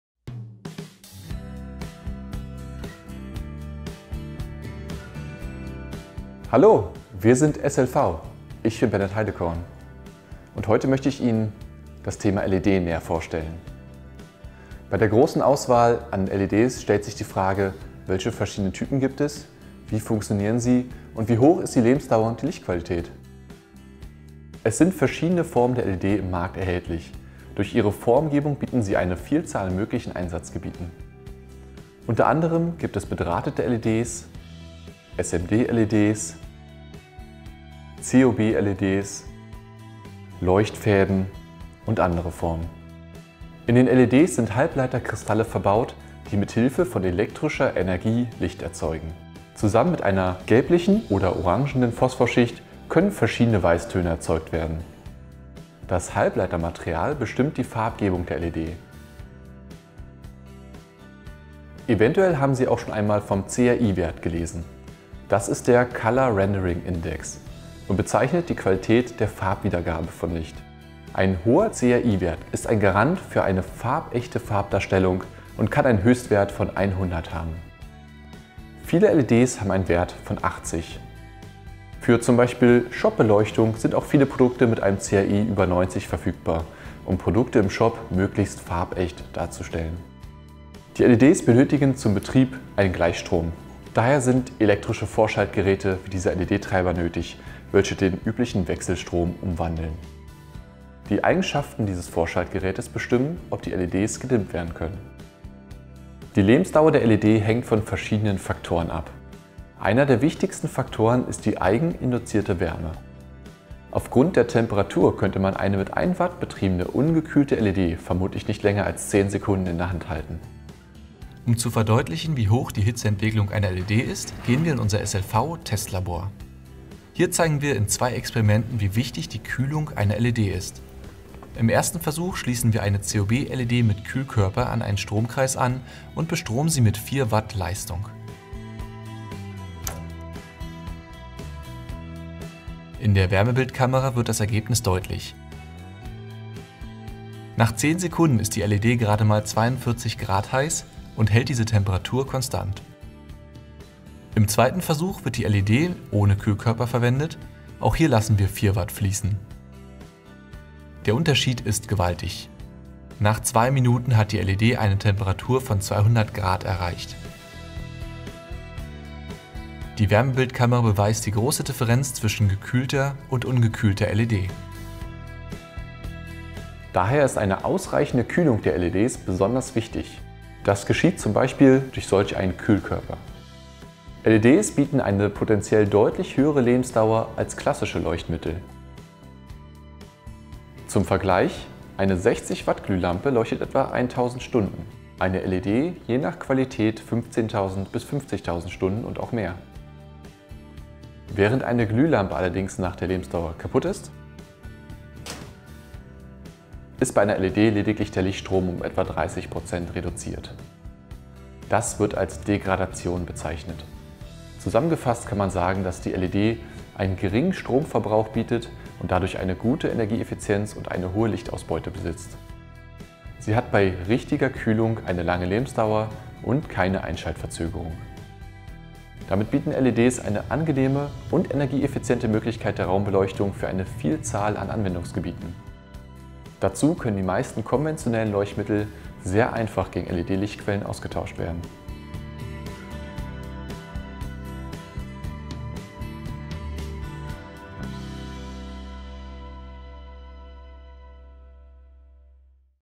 SLV - Macht schlau | LED Technik
LED ist die Zukunft des Lichts: unendliche Vorteile, unbegrenzte Einsatzmöglichkeiten und Beleuchtungslösungen, für die Nachhaltigkeit und Energiesparen selbstverständlich sind.

Aber welche Eigenschaften haben LEDs überhaupt und welche Technik steckt dahinter? Und wie funktioniert eine LED?

Wir zeigen die wichtigsten Fakten rund ums Thema LED:

➨ Aufbau und Funktion
➨ verschiedene Formen
➨ Lebensdauer
➨ Farbwiedergabeindex (CRI)
➨ Wärmeentwicklung

Entdecken Sie unsere große Auswahl an hochwertigen LED-Produkten und modernen Beleuchtungsmöglichkeiten ➽